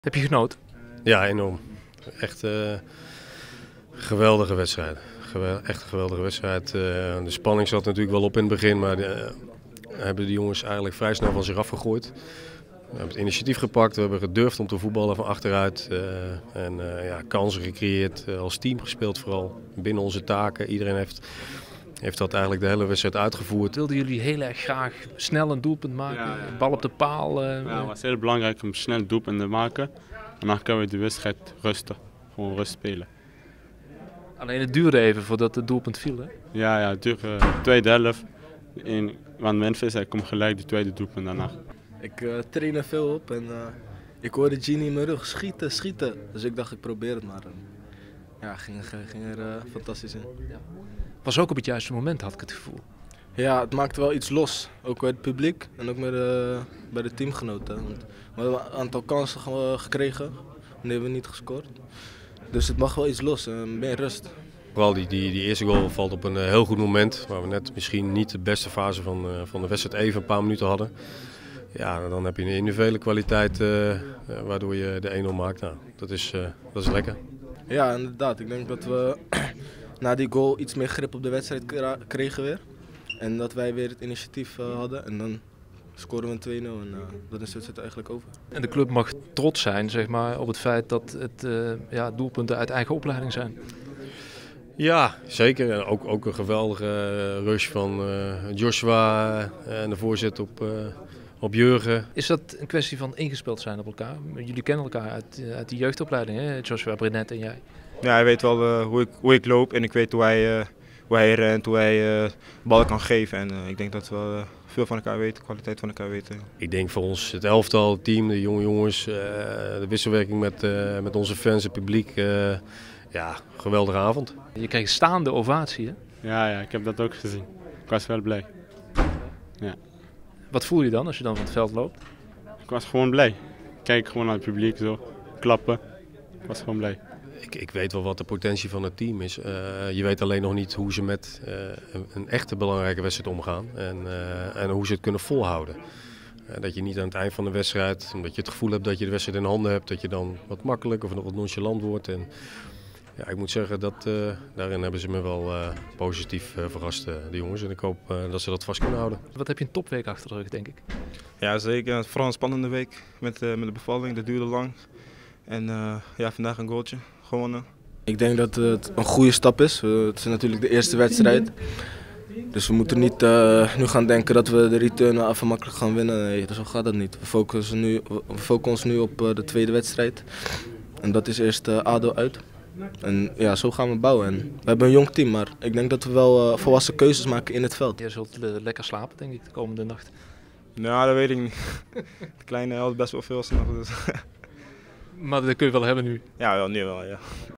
Grote grijns na Europese start
Het jeugdige en talentvolle PSV heeft zich laten zien aan de voetbalwereld. Vol enthousiasme won de ploeg van Phillip Cocu van Zulte Waregem. De pas 17-jarige Bakkali, en doelpuntenmakers Depay en Locadia straalden na afloop van oor tot oor.